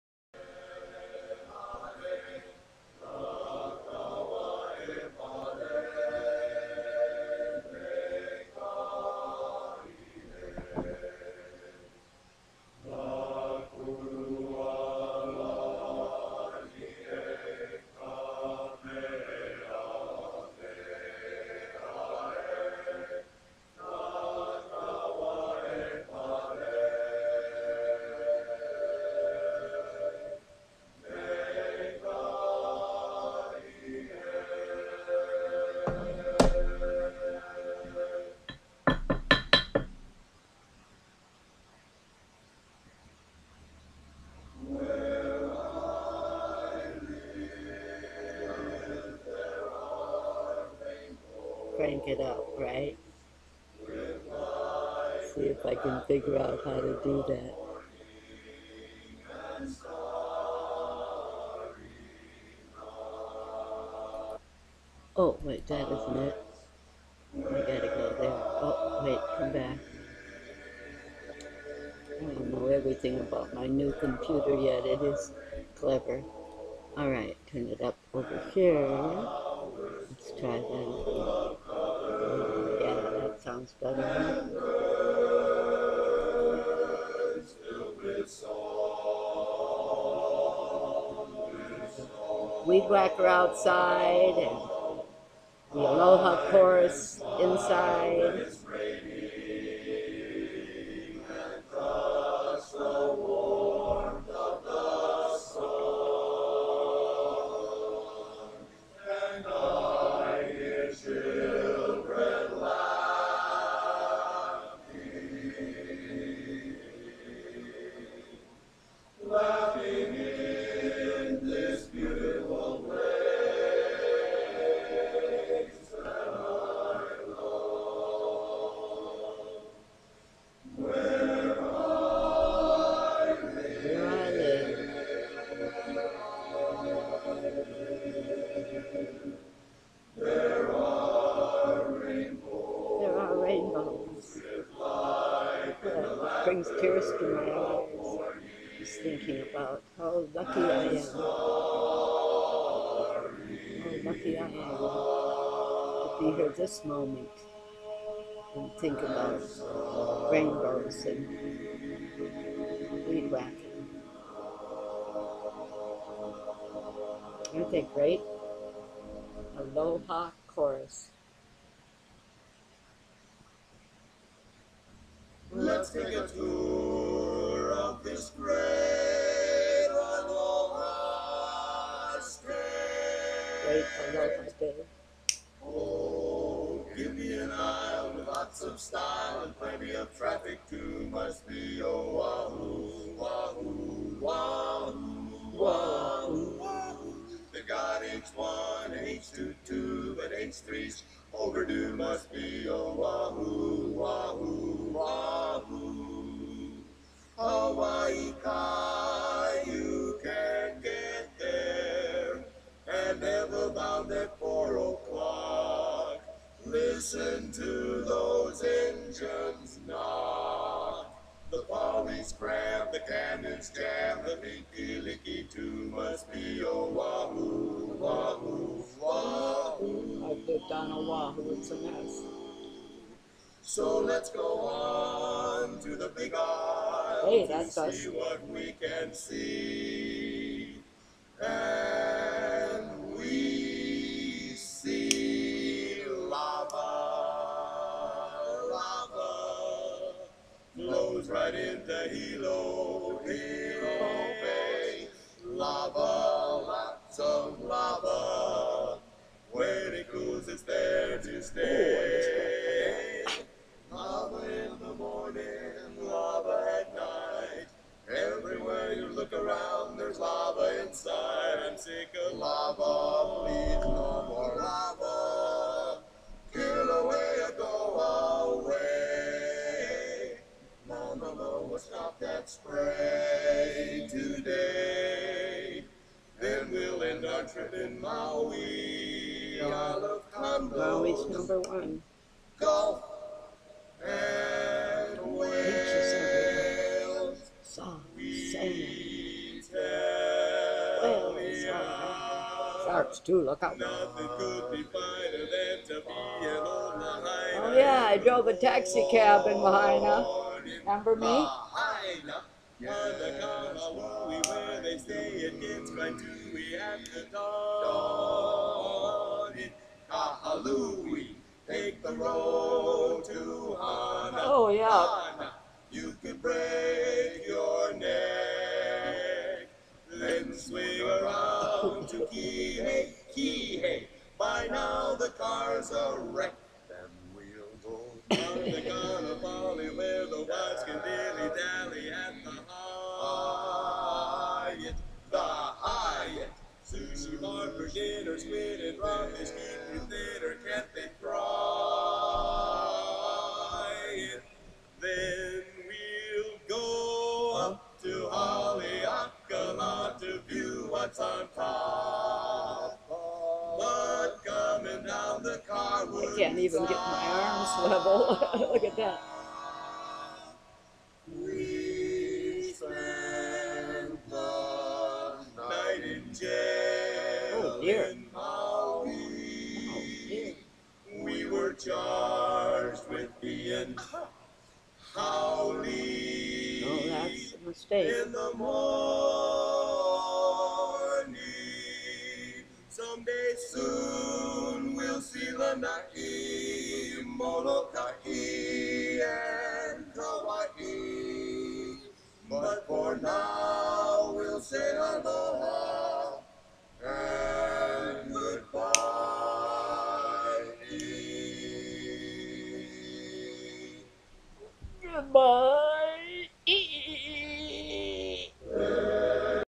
MAY 14 11 GRANDMA NAN PLAYS ALOHA CHORUS.wmv
GRANDMA NAN PLAYS THE ALOHA CHORUS ... THE WONDERFUL MUSIC OF THE BARBERSHOP QUARTET ON HAWAII ... YOU CAN BUY THEIR CD ON AMAZON OR DIRECTLY FROM THEM.